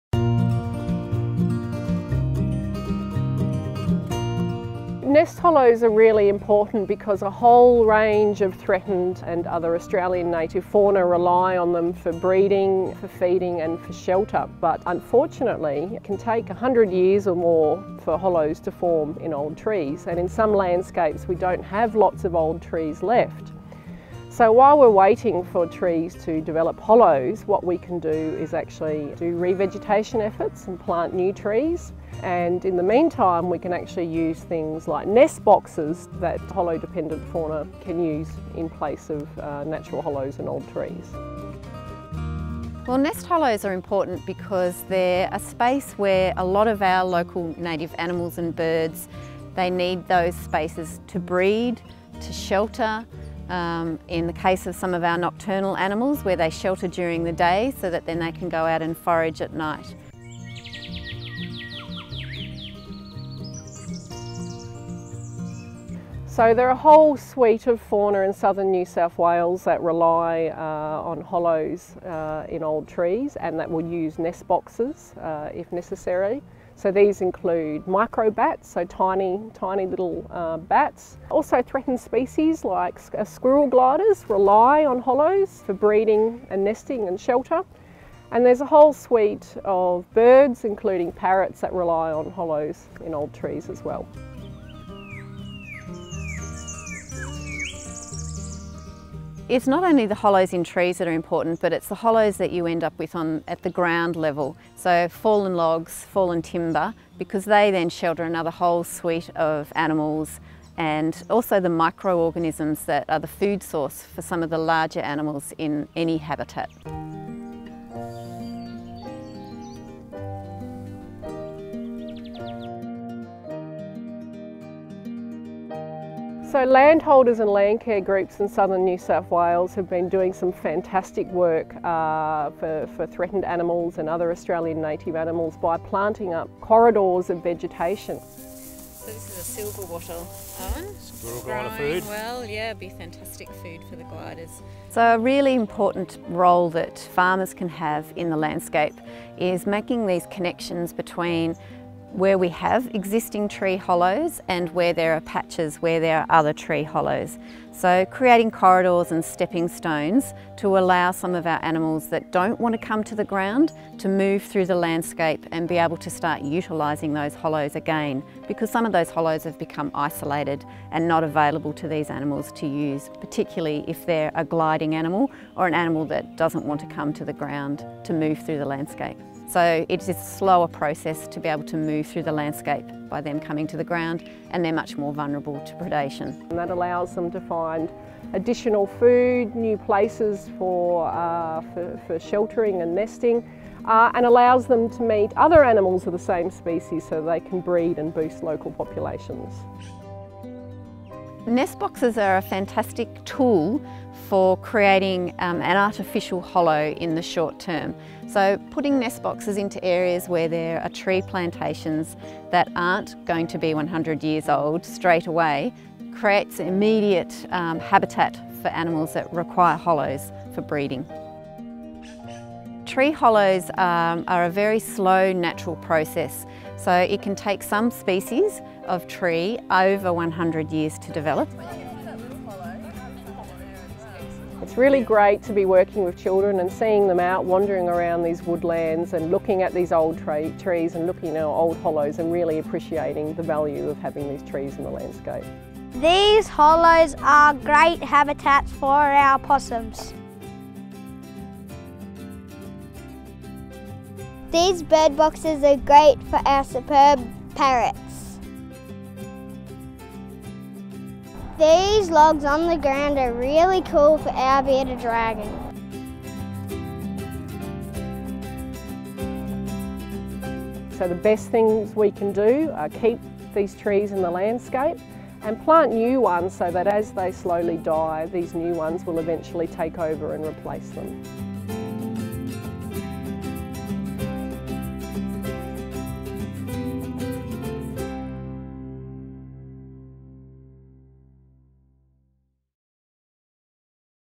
Nest Hollows in Southern NSW
Nest hollows are extremely important for Australian native wildlife for breeding, feeding and shelter. Unfortunately it can take up to 100 years for hollows to form in old trees and in some landscapes we don't have lots of old trees left.

Produced by Murrumbidgee Landcare Inc., Riverina Local Land Services and Petaurus Education Group.